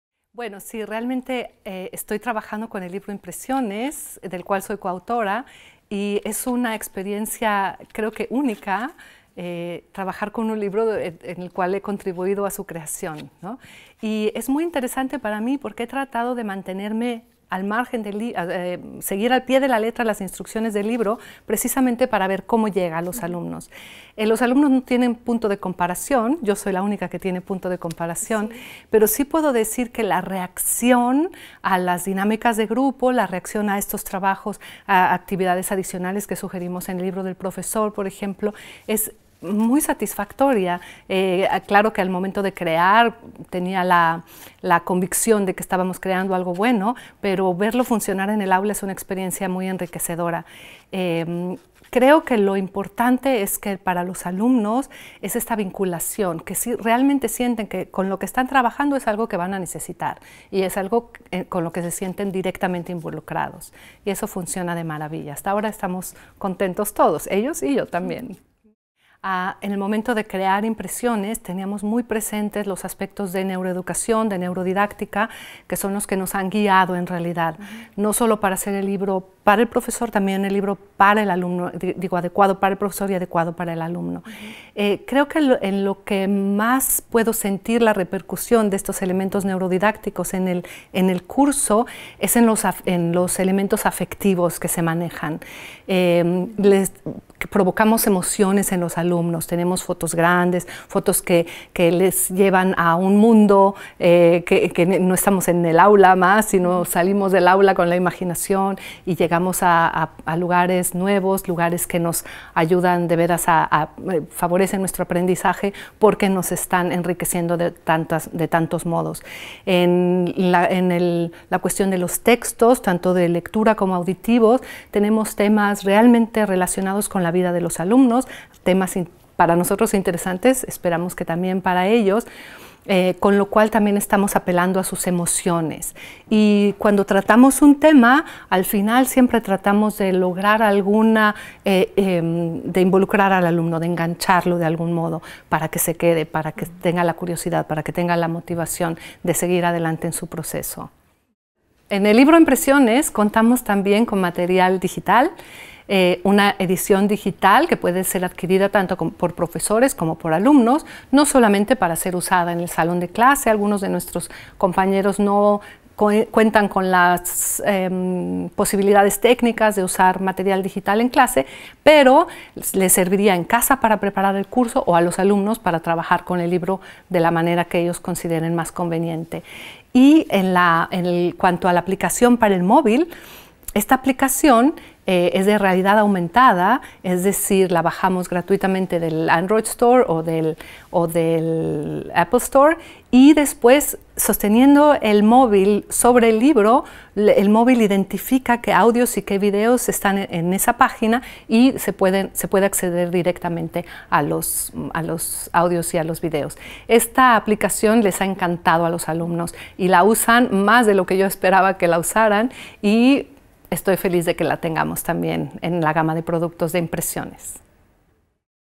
Interview mit Claudia Teissier de Wanner, Congreso de Español de Hueber 2017
Interview mit Claudia Teissier de Wanner im Rahmen des Congreso de Español de Hueber 2017 in München. Weitere Informationen und Materialien für Ihren Spanisch-Unterricht finden Sie